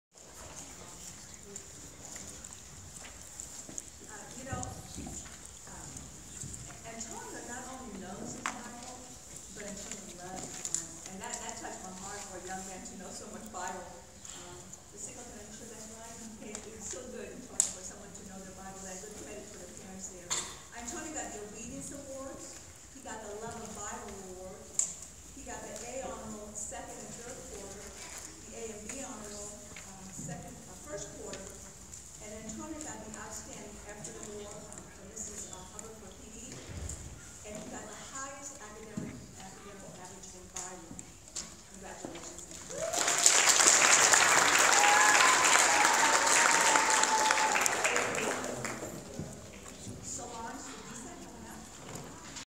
Antonio graduating 4th grade with high honors. Providence Christian School. May 24, 2017
Providence Christian School, Riverview, Florida